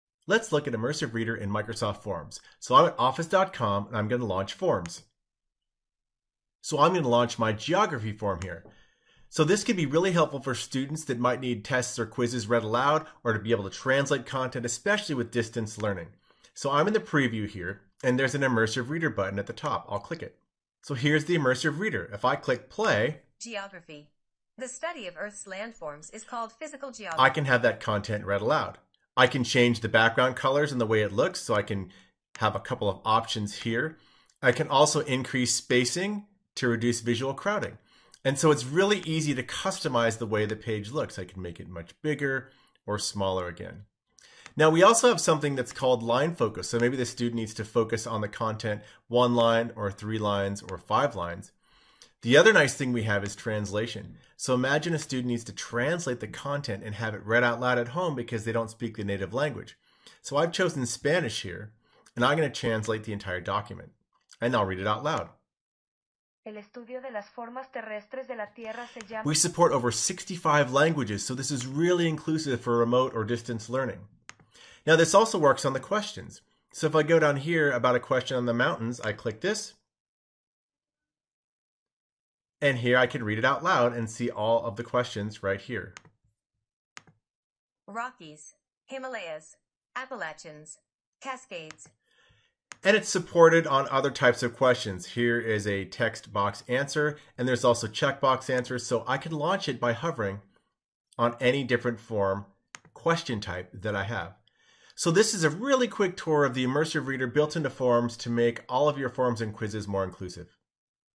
How to use Immersive Reader in Microsoft Forms
A step-by-step tutorial on how to use Immersive Reader in Microsoft Forms to allows your forms to be more inclusive and accessible. The Immersive Reader in Microsoft Forms Helps with reading, translation, and other inclusive reading features built right in and all free

📺 For a Microsoft Forms tutorial and lots of tips and tricks, check out my Forms playlist